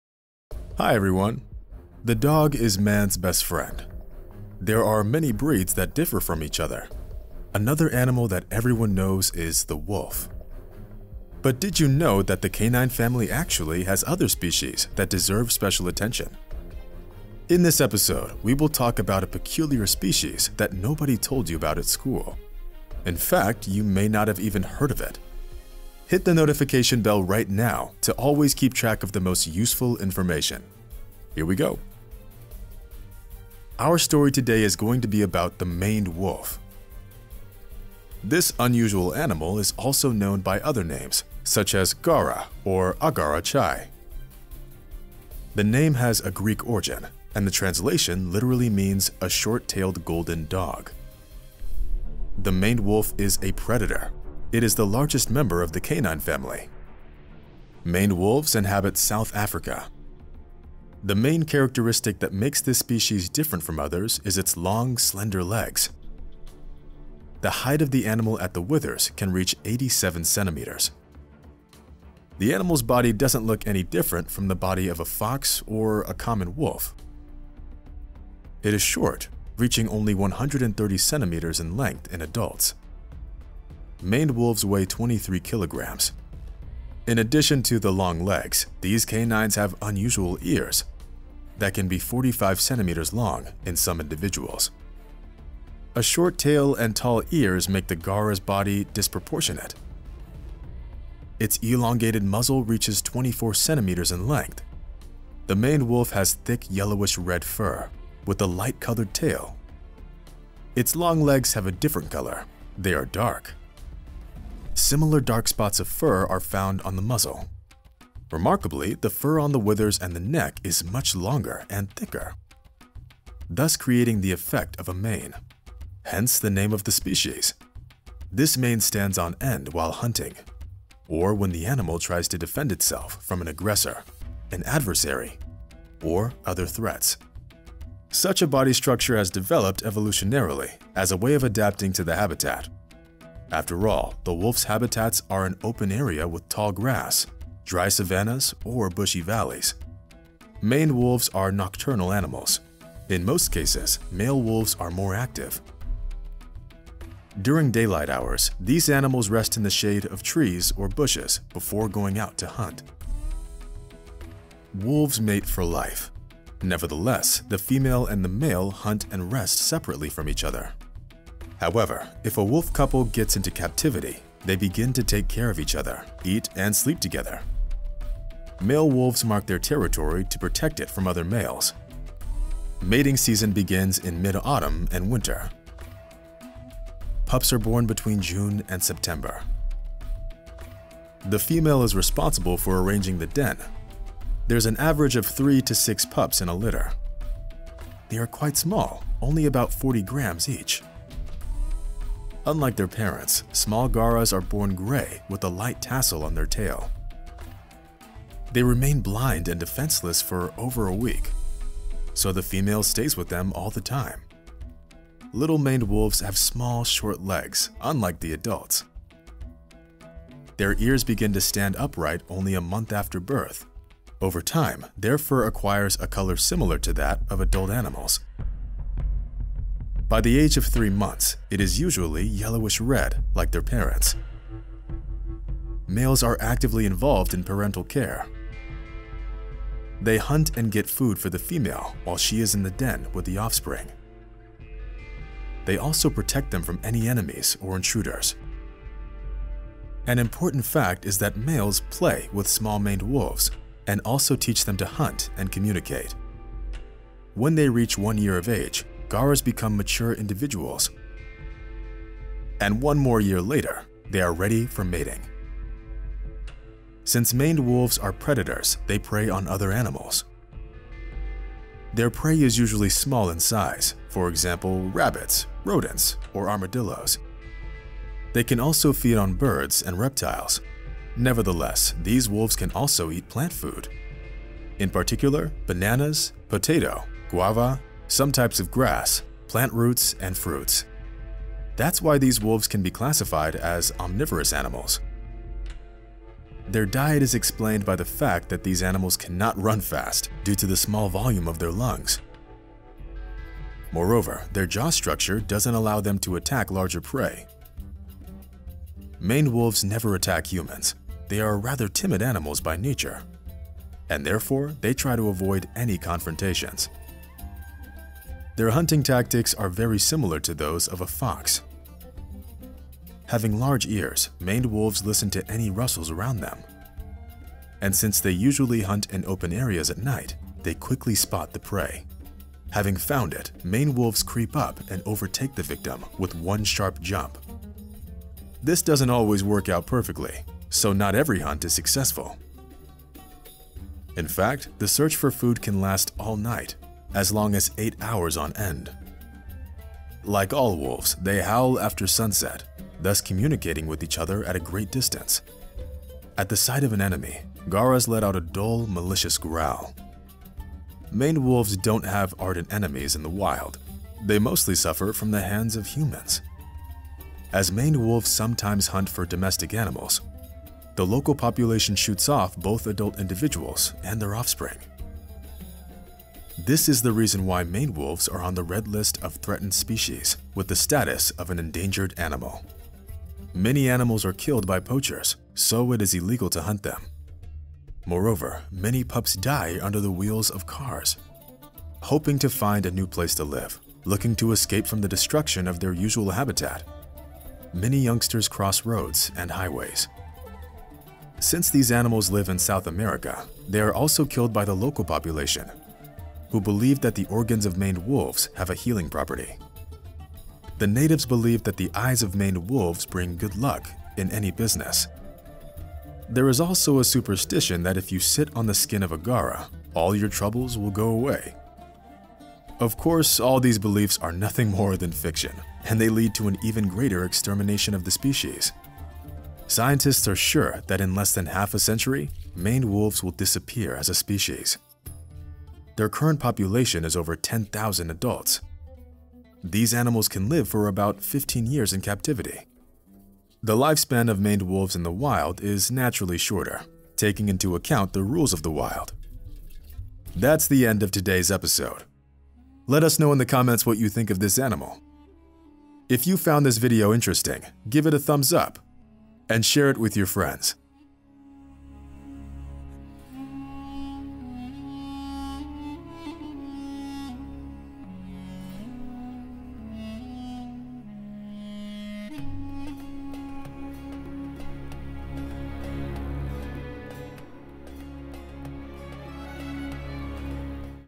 The Unique Predator - A Maned Wolf
The dog is man's best friend. There are many breeds that differ from each other. Another animal that everyone knows is the wolf. But did you know that the canine family actually has other species that deserve special attention? In this episode, we will talk about a peculiar species that nobody told you about at school. In fact, you may not even have heard of it. Hit the notifications bell right now to always keep track of the most useful information.